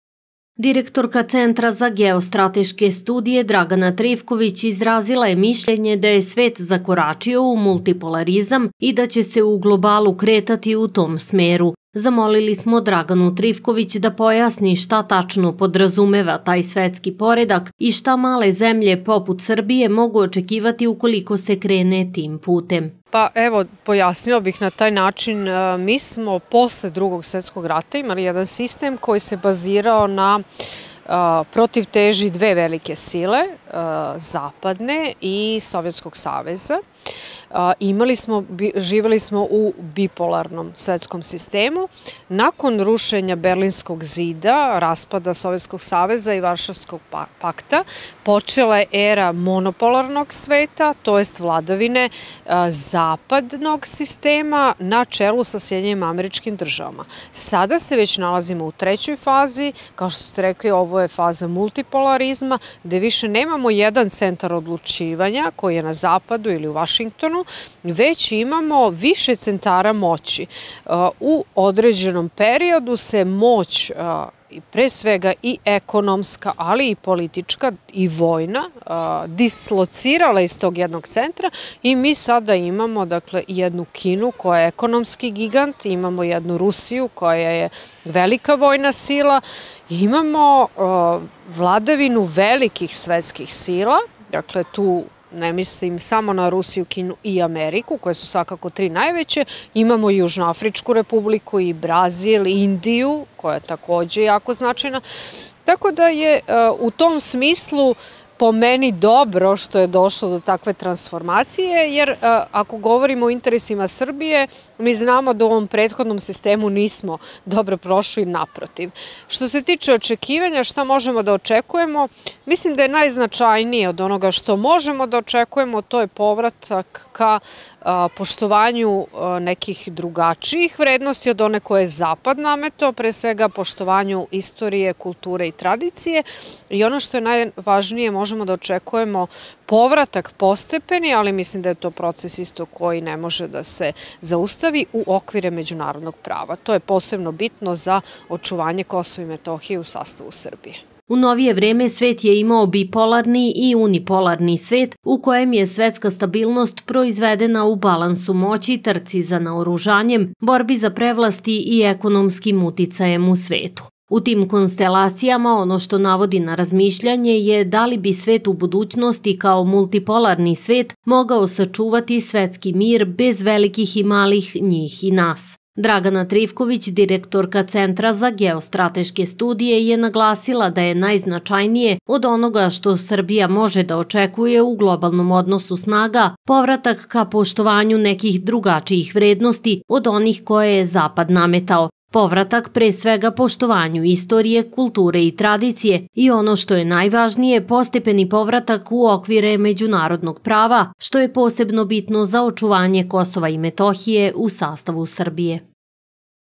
Dragana Trifković o položaju Srbije u međunarodnim odnosima
U komplikovanim međunarodnim odnosima Srbija pokušava da se snađe u svetu kojim rukovode velike globalne sile poput Amerike, Rusije i Kine. Jedan od glavnih spoljnopolitičkih ciljeva Srbije je očuvanje Kosova i Metohije u njenim granicama, u čemu joj najviše podršku pružaju Rusija i Kina.